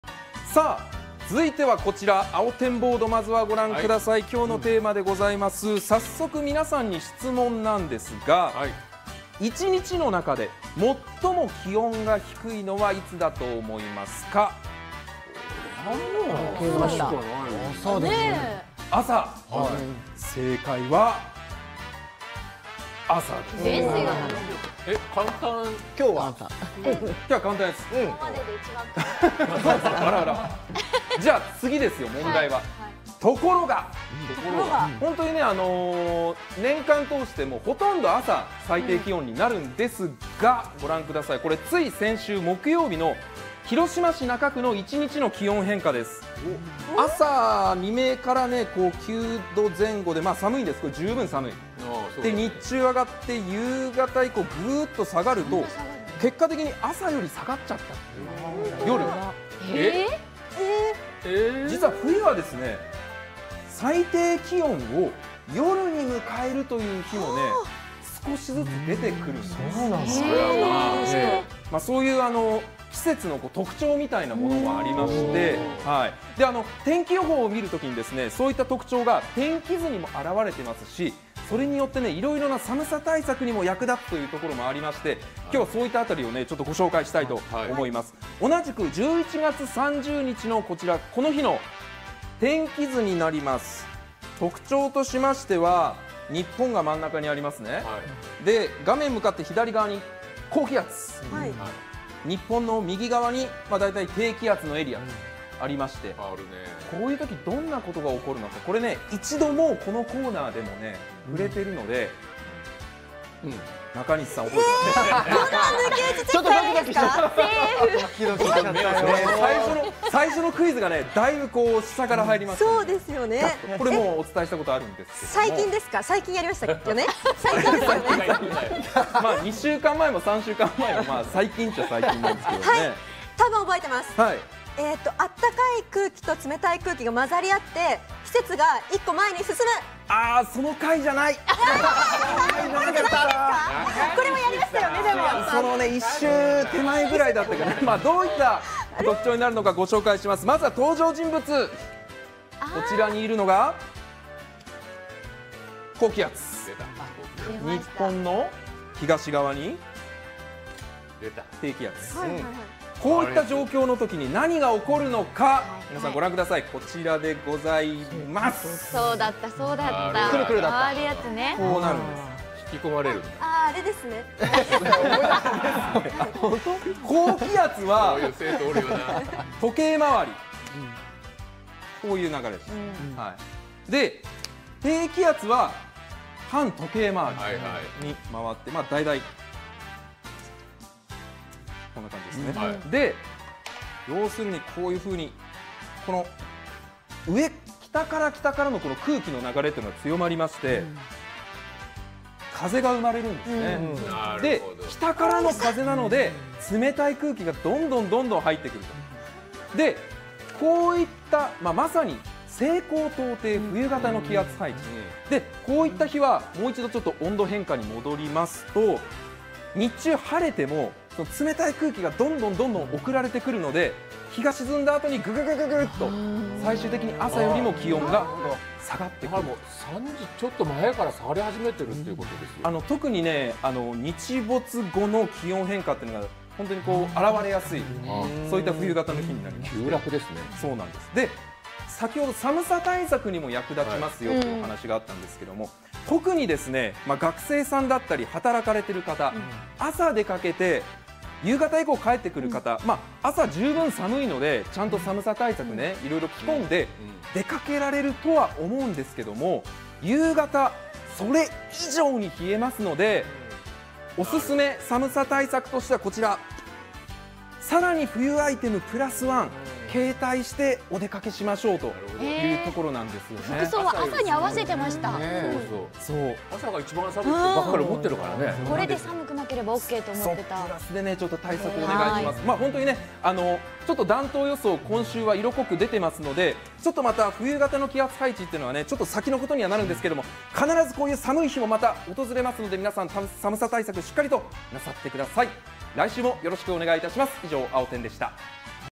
【あおテン】「1日の中で最も気温が低いのは朝…しかし11/30は夜が最低気温に!?」最低気温の仕組みを TSS青坂気象予報士が分かりやすく解説
TSS青坂気象予報士が天気に関する情報を分かりやすく解説！
「1日の中で最も気温が低いのは朝…しかし11/30は夜が最低気温に!?」最低気温の仕組みを TSS青坂気象予報士がレクチャーします！